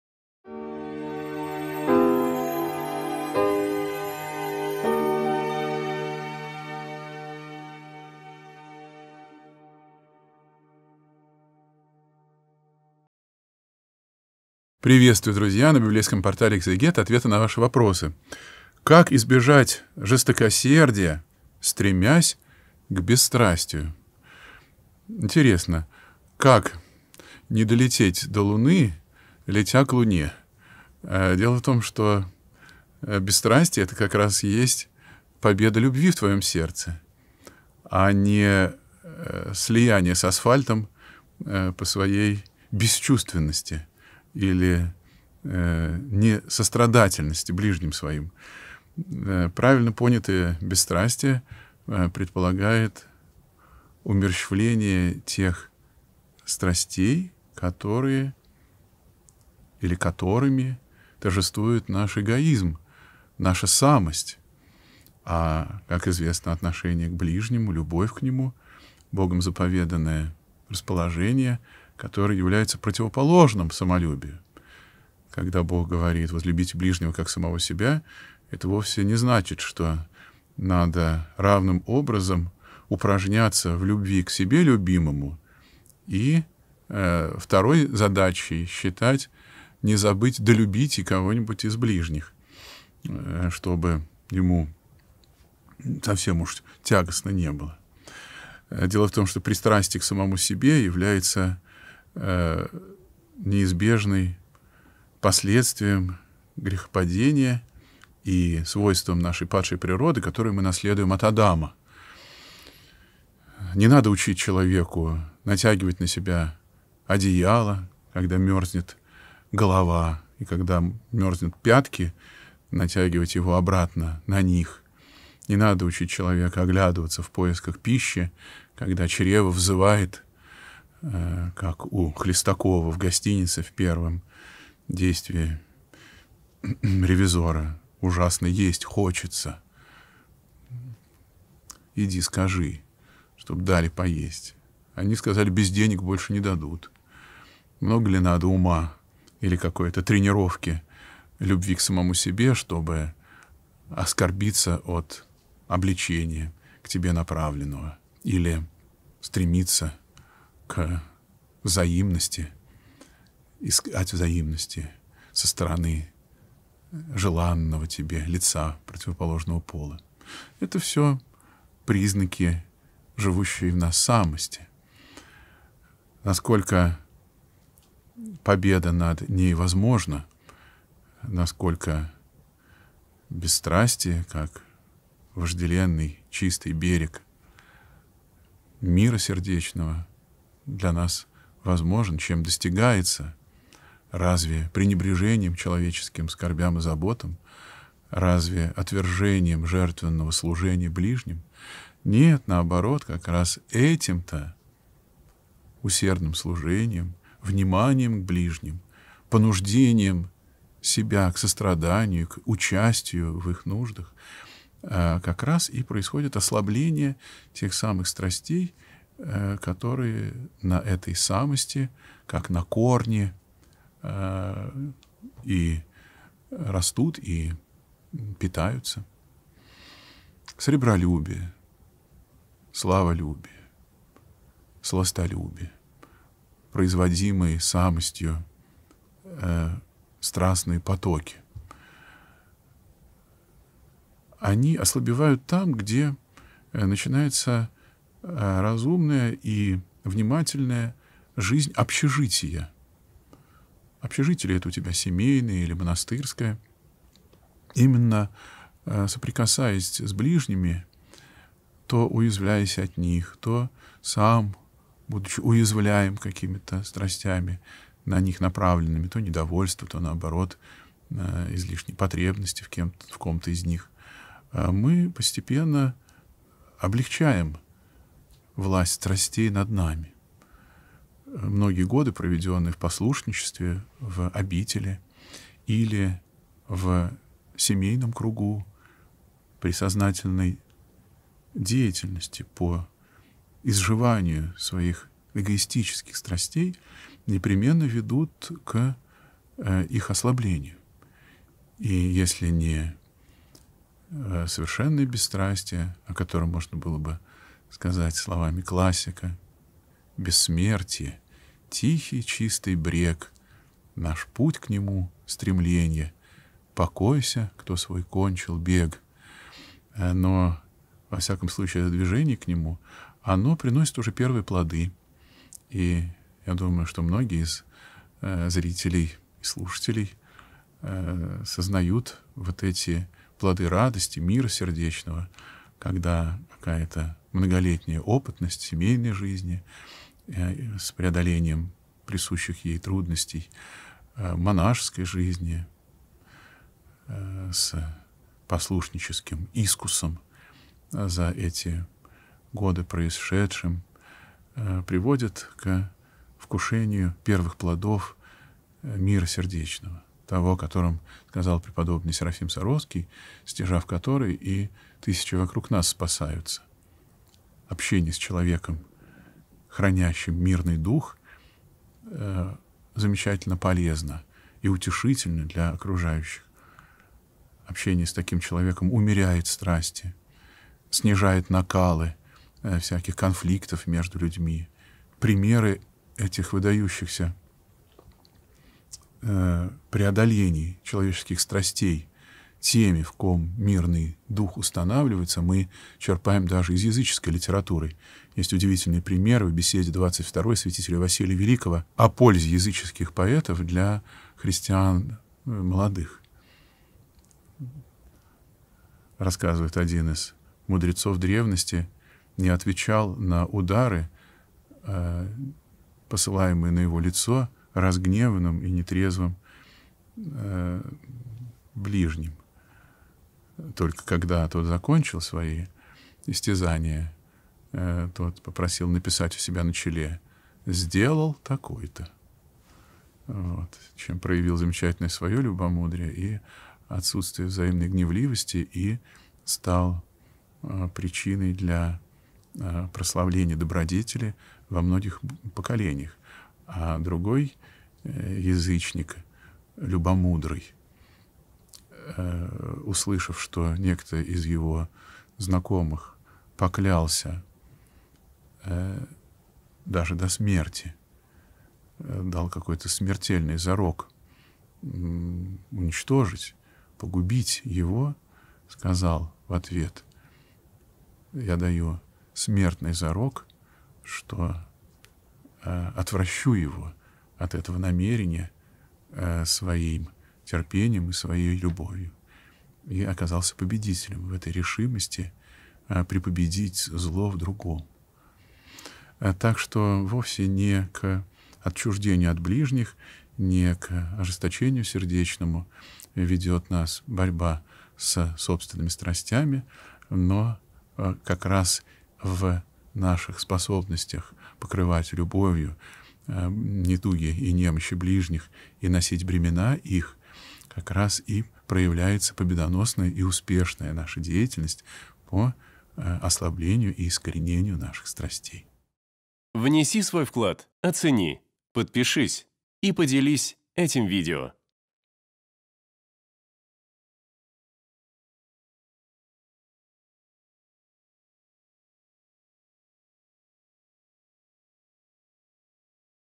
Как не стать ЖЕСТОКОСЕРДНЫМ, стремясь к БЕССТРАСТИЮ ? Игумен Лука (Степанов)
Команда Экзегет.

В сегодняшнем видео на библейском портале Экзегет игумен Лука (Степанов) отвечает на вопрос: "Как не стать жестокосердным, стремясь к бесстрастию?"

🔸  Бесстрастие – победа любви в сердце
🔸  Умерщвление страстей  самости
🔸  Пристрастие к самому себе
🔸  Усердное служение ближнему
🔸  Понуждение себя к состраданию
🔸  Ослабление власти страстей
🔸  Мир сердечный
🔸  Плоды радости